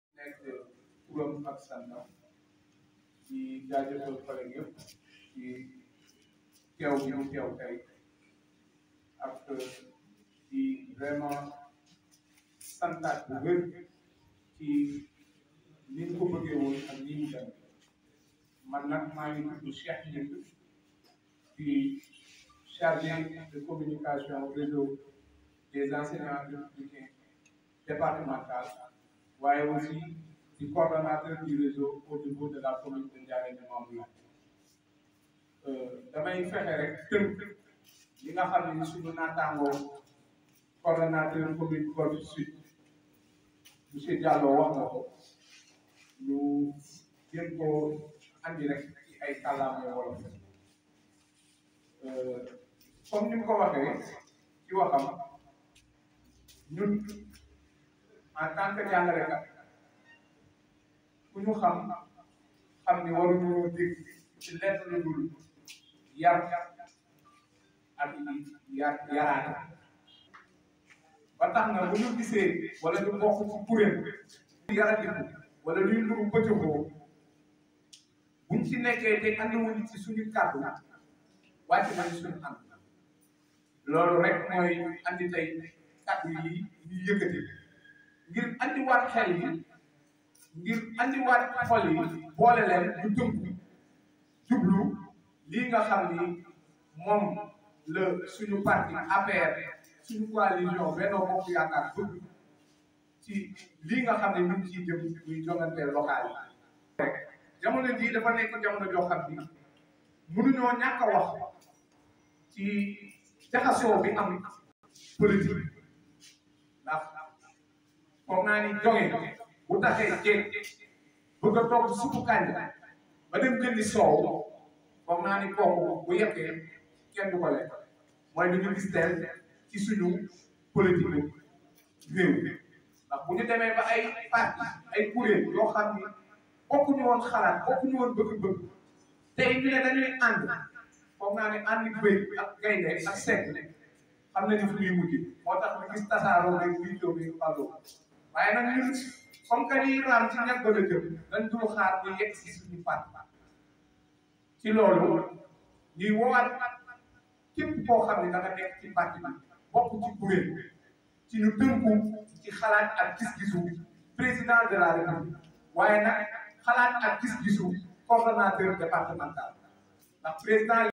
10 septembre 2021
Locales à Golf/Sud( Guédiawaye) :  Lat Diop, le choix des enseignants Républicains...

Les manifestations de soutien en faveur de Lat Diop continuent de fleurir au niveau de la Commune de Golf/Sud. Après la COJER, les liberaux, etc. , c'est autour,  ce vendredi de la branche locale du Réseau des Enseignants Républicains de rendre publique leur choix. Les animateurs de cette structure di parti présidentiel ont fixé leur dévolu sur le Directeur Général de la LONASE pour conduire la liste communale lors des prochaines élections municipales.